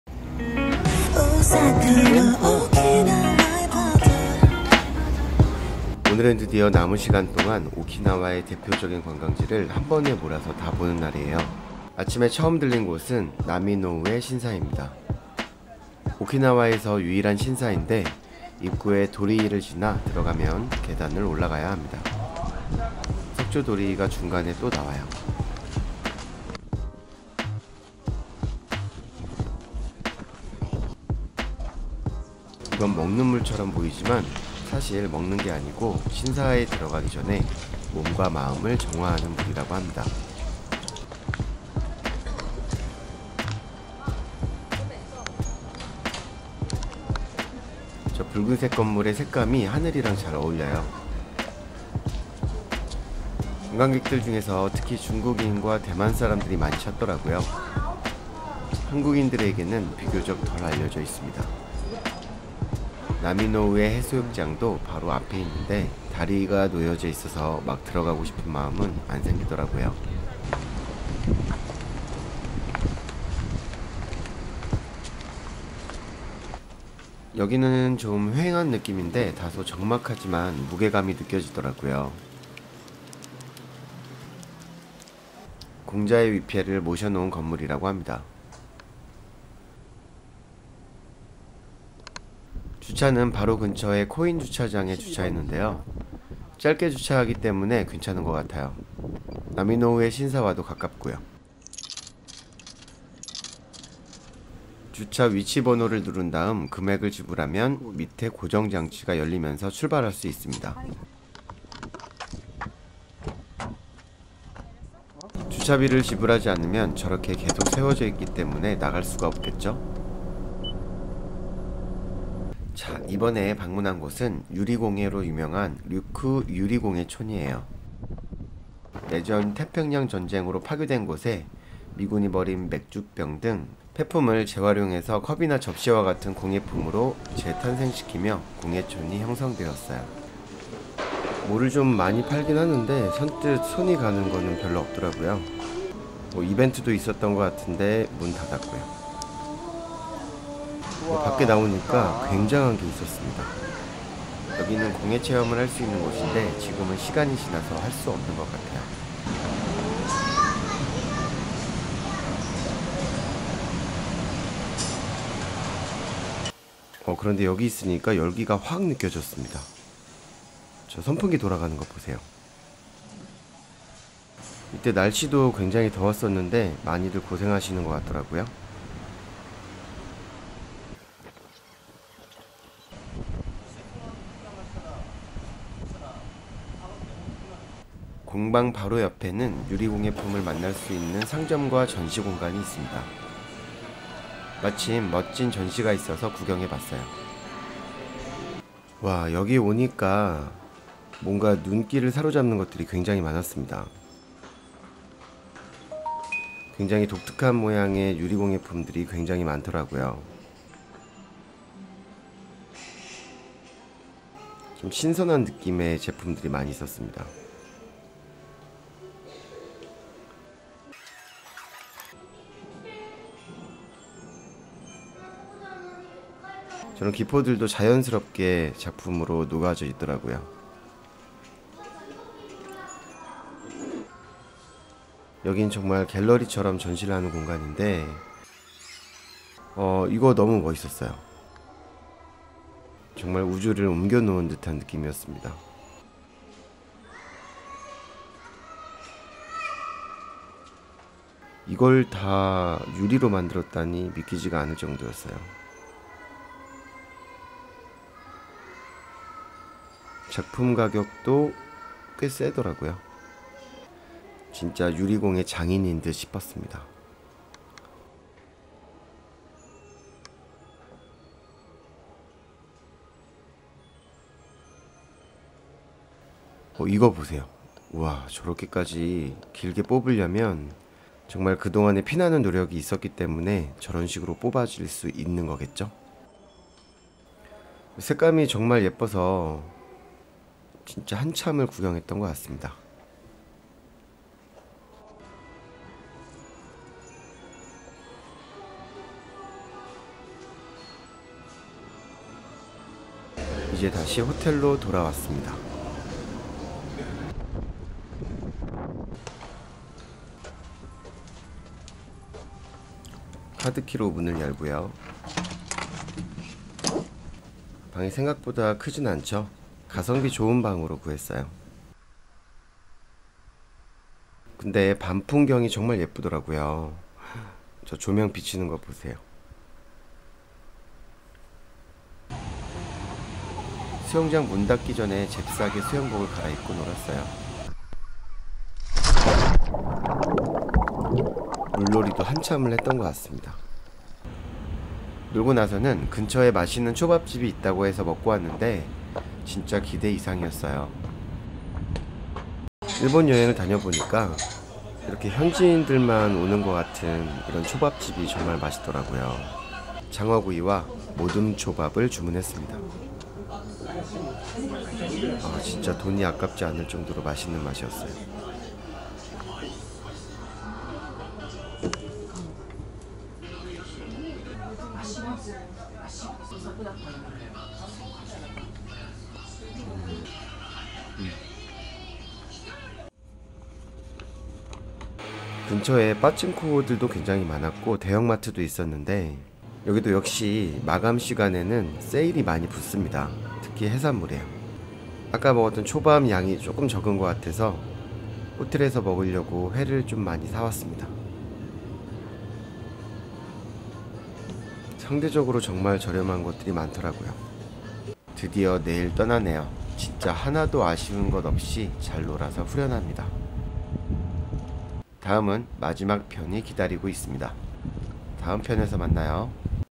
오키나와 필수 여행지 맛집, 호텔 모음.ZIP
오늘은 드디어 남은 시간동안 오키나와의 대표적인 관광지를 한번에 몰아서 다 보는 날이에요. 숙소도 오늘만은 수영장도 있는 좀 더 좋은 곳으로 예약해서 마지막날의 유종의 미를 찍어보려고 합니다.

오키나와 필수 여행지와 맛집 호텔 모음집 함께 보실까요?